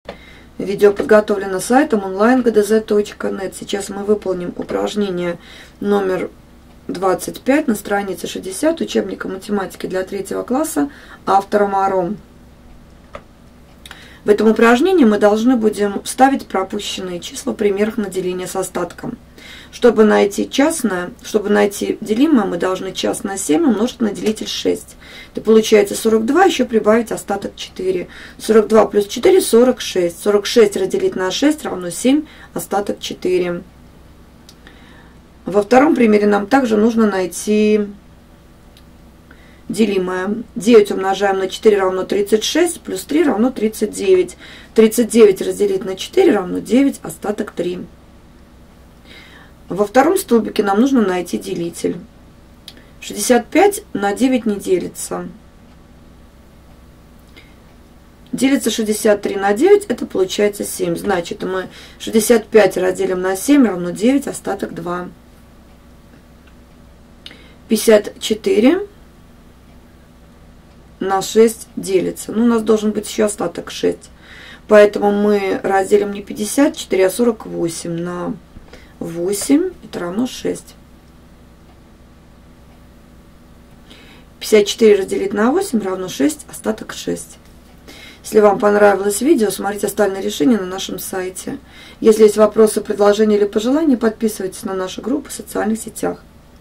Страница 60 Задание 25 – Математика 3 класс (Моро) Часть 2
Чтобы выполнить задание №25 на странице 60, вы должны уметь вставлять пропущенные числа, выполнять деление двузначных чисел на однозначные числа с остатком. Обращайтесь к нашему видео-решению, если хотите выполнить домашнее задание на отлично.